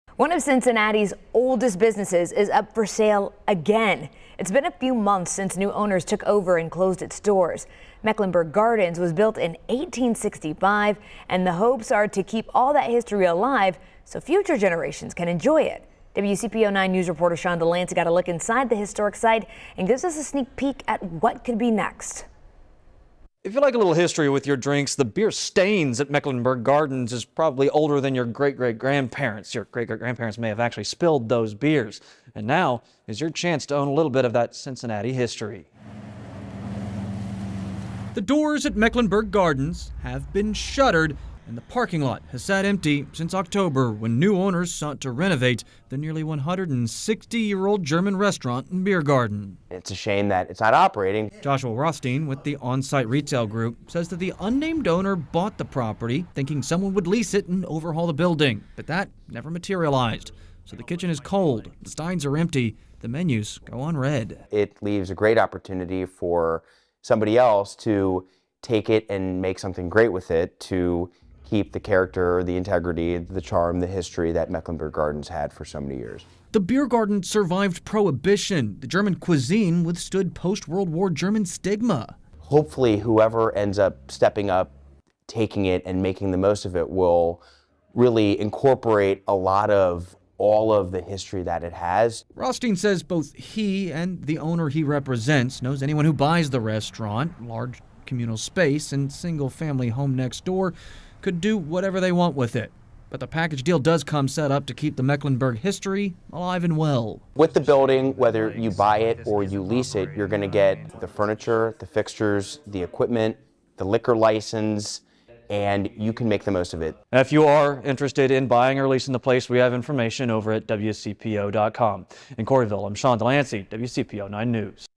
Historic German restaurant up for sale, or lease, once again
Months after falling under new ownership and closing its doors in October, Mecklenburg Gardens is once again being offered on the market.